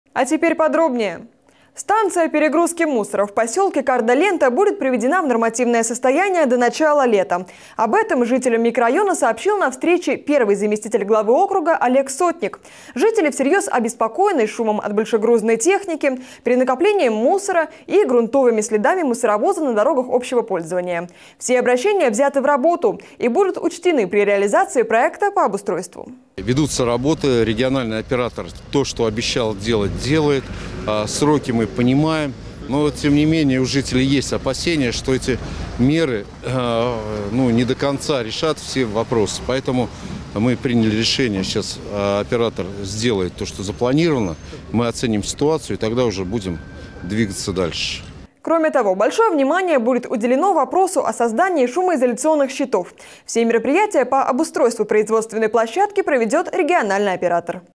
Производственная площадка в п. Кардо-Лента будет обустроена до 1 июня
Станция перегрузки мусора в посёлке Кардо-Лента будет приведена в нормативное состояние до начала лета. Об этом жителям микрорайона сообщил на встрече первый заместитель главы округа Олег Сотник. Жители всерьёз обеспокоены шумом от большегрузной техники, перенакопления мусора и грунтовыми следами мусоровозов на дорогах общего пользования. Все обращения взяты в работы и будут учтены при реализации проекта по обустройству.  (12+)